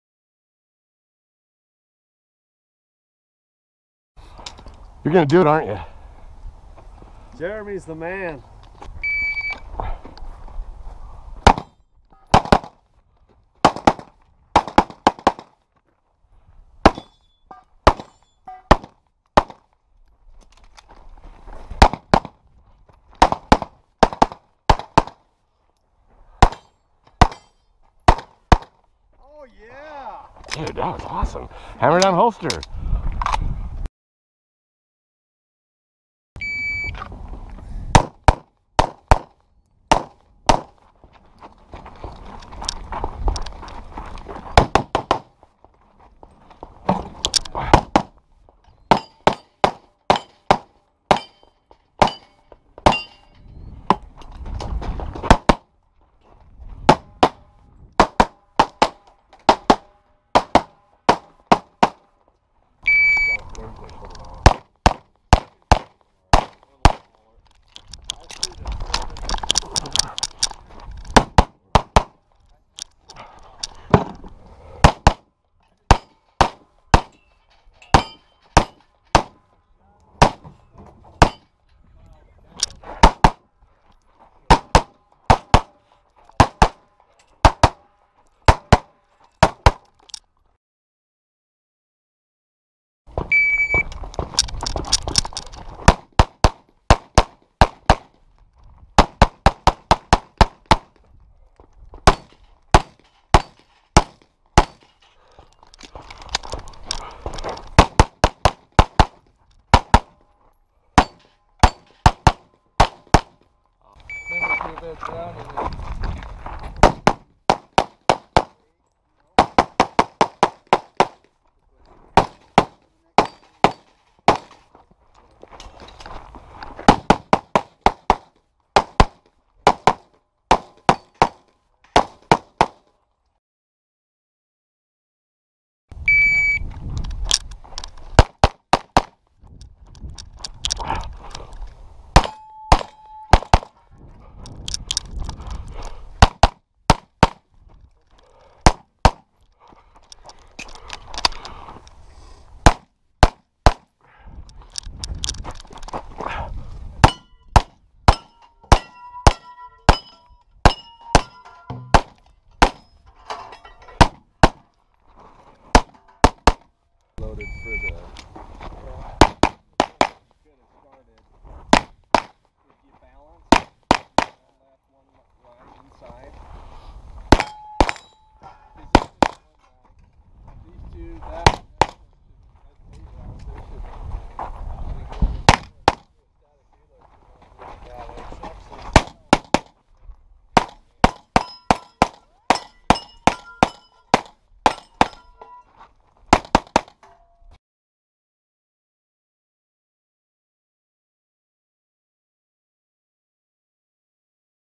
2016 USPSA CAPS Club Practical Pistol Shooting Sports Match Competition
A few terrific stages.  Jeremy Allen threads the needle,  with Russ Reeder, Kirby Lunt, and Mark Brown.  utahshooters.org